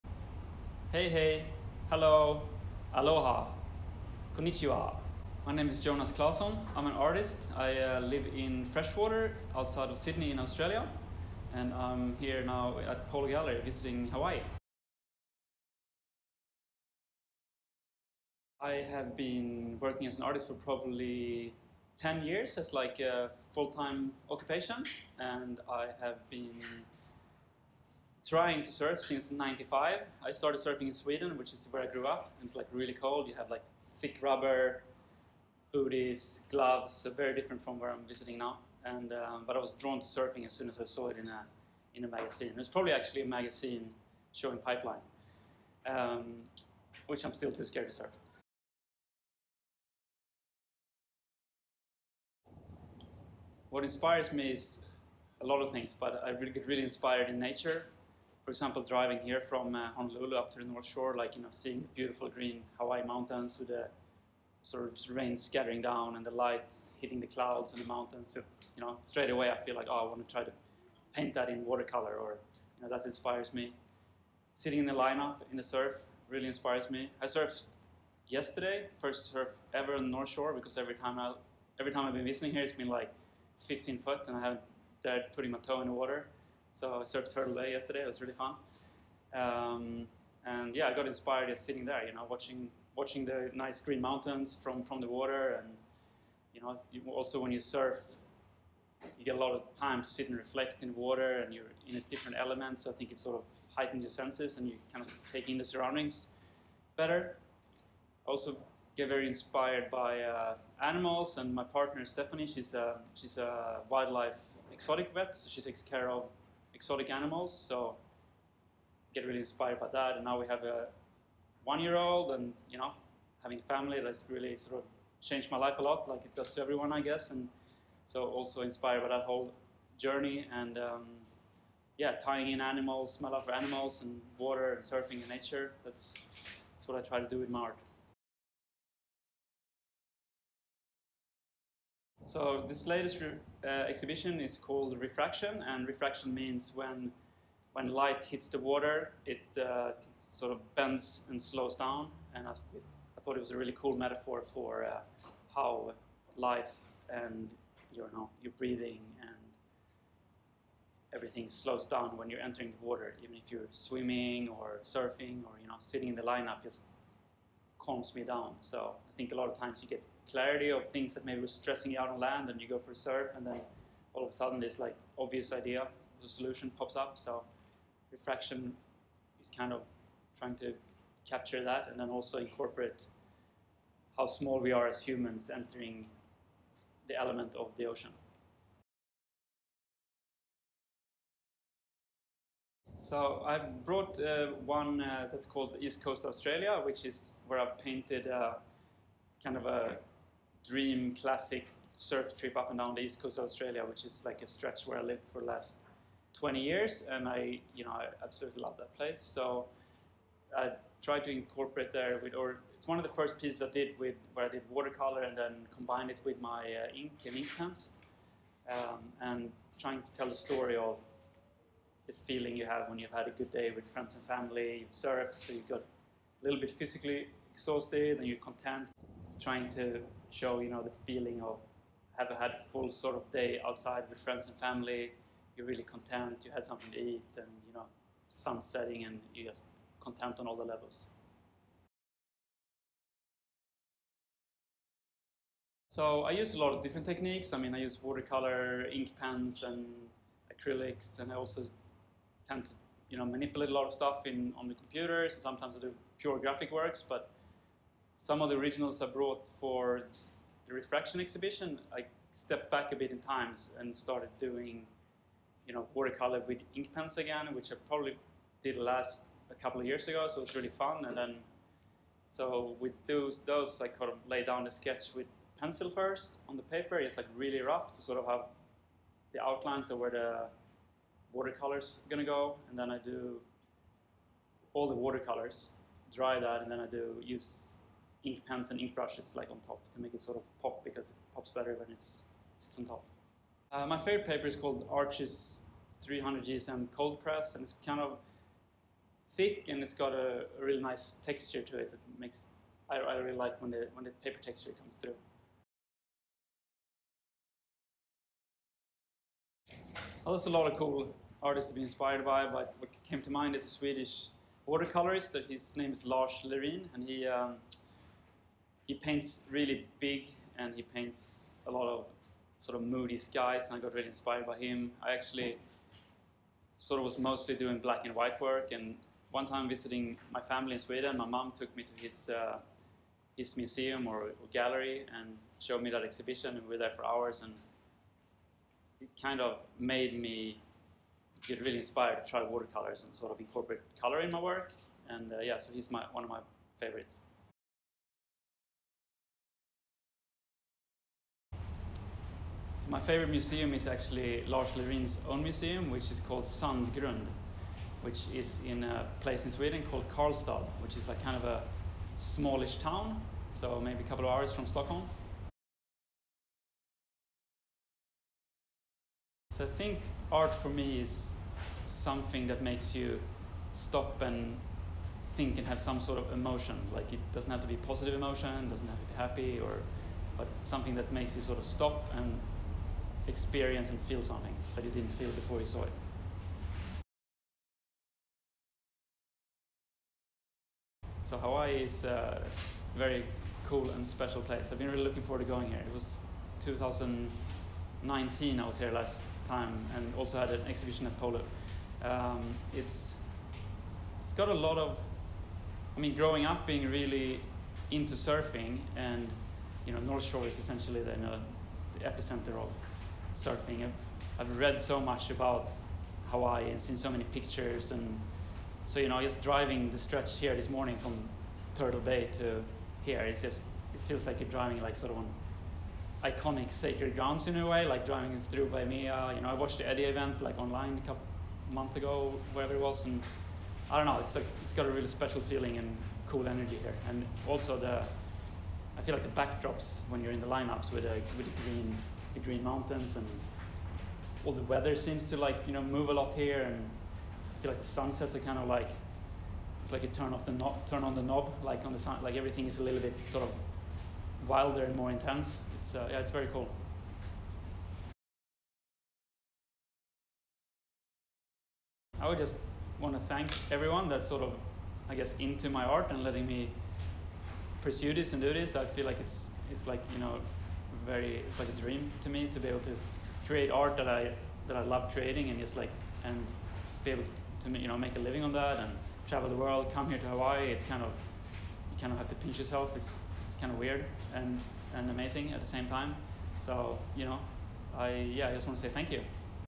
Jonas Claesson Interview 2023 Polu Gallery
Jonas is one of the most famous artists in the Polu Gallery. His work "Whale Shark" now reminds thousands of art lovers of the glory and stillness of the ocean in the comfort of their homes.

His book "Surfing Animals Alphabet" shows his true spirit as a surfer and creative, skilled to put his two loves, art and surfing, together.

Jonas Claesson’s Exhibition Opening Reception in Polu Gallery Hale’iwa was great. Many fans of Jonas’s art met with him, took pictures, and supported his art. Mahalo to everyone who stopped by on this special day for our gallery.